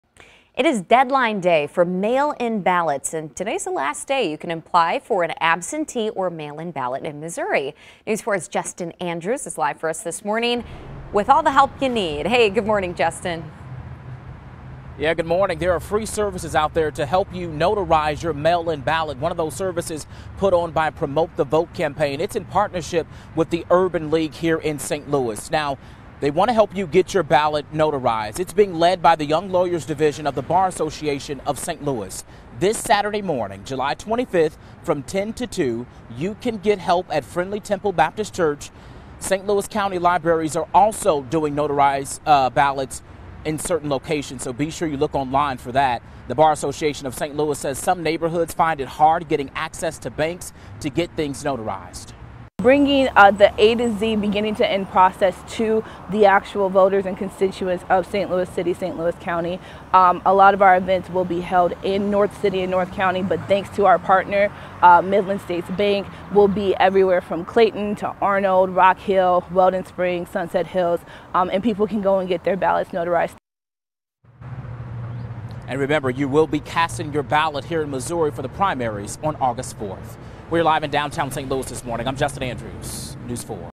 St. Louis Bar Assocaition helping assist with mail-in ballots
The St. Louis Bar Association, in conjunction with the Urban League of Metropolitan St. Louis is creating a Promote the Vote campaign aimed to assist the community with getting mail-in ballots notarized.